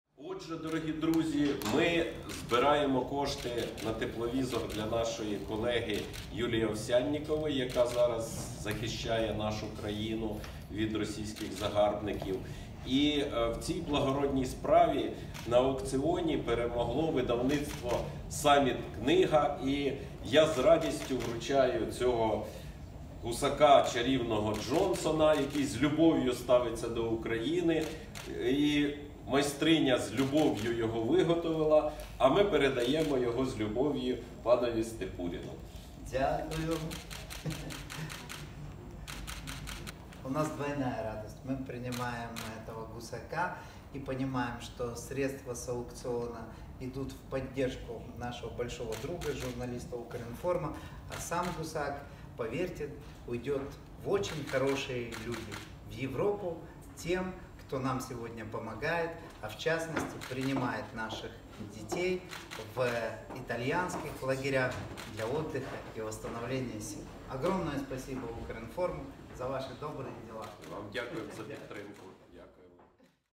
На тепловізор для фотокора: Гусак Джонсонюк пішов з аукціону за 10 тисяч
Зібрані кошти будуть перераховані на купівлю тепловізора для нашої колеги - фотокореспондента Юлії Овсяннікової.
Сьогодні у приміщенні інформаційного агентства Укрінформ генеральний директор Олександр Харченко вручив головний лот аукціону, лялькову копію британського прем'єра, переможцю - директору видавництва "Саміт-книга" Івану Степуріну.

"Як ви знаєте, ми збираємо гроші на тепловізор для нашої колеги, фотокореспондента Укрінформу Юлії Овсяннікової, яка зараз захищає нашу країну від російських загарбників. У цій благородній справі, на благодійному аукціоні, перемогло видавнцитво "Саміт-книга" - і я з радістю вручаю цього гусака, чарівного Джонсона, прототип якого з любов'ю ставиться до України, і який майстриня виготовила з любов'ю, пану Степуріну", - зазначив Олександр

"У нас подвійна радість: ми приймаємо цього гусака і розуміємо, що кошти від аукціону йдуть на підтримку нашого великого друга, журналіста Укрінформу, а сам гусак, повірте, піде в дуже хороші люди - в Європу, до тих, хто нам сьогодні допомагає, зокрема, приймає наших дітей в італійських таборах для відпочинку і відновлення сил. Велике дякую Укрінформу за ваші добрі справи", - сказав у свою чергу директор видавництва Іван Степурін.